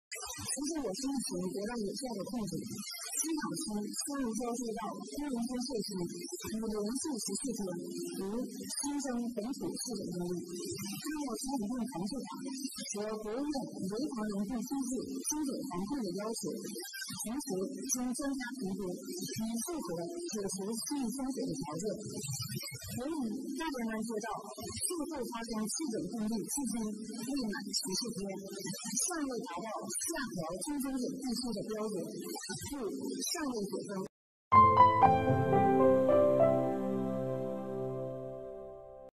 大连西岗区工人村社区解除封闭管理
8月7日，大连市卫生健康委副主任赵连称，随着大连疫情得到有效控制，西岗区香炉礁街道工人村社区已连续14天无新增本土确诊病例，按照《传染病防治法》和国务院联防联控机制精准防控的要求，同时经专家评估，已符合解除区域封锁的条件。